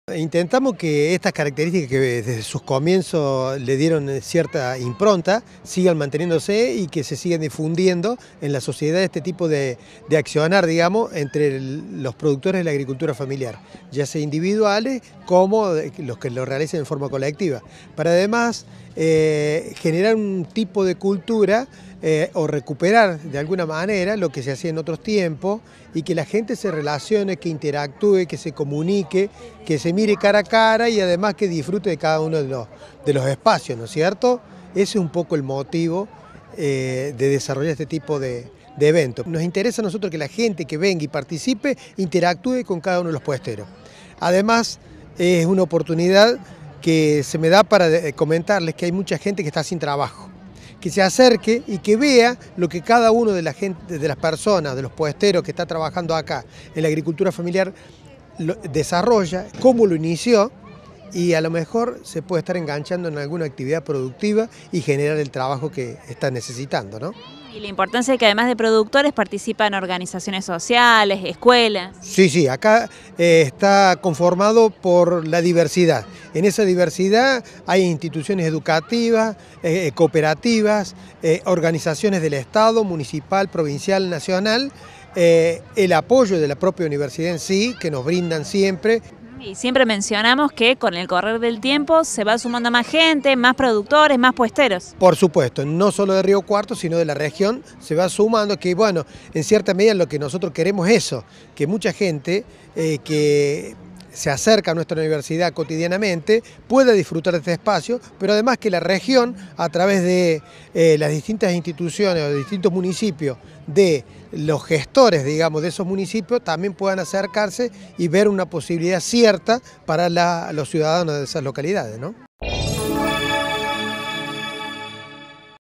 NUEVA EDICIÓN DE LA FERICAMBIO EN LA UNRC - OSVALDO DIEZ – COORDINADOR
PARTICIPARON PRODUCTORES, ORGANIZACIONES E INSTITUCIONES EDUCATIVAS
Facebook.com/uniriotv
Twitter.com/uniriotv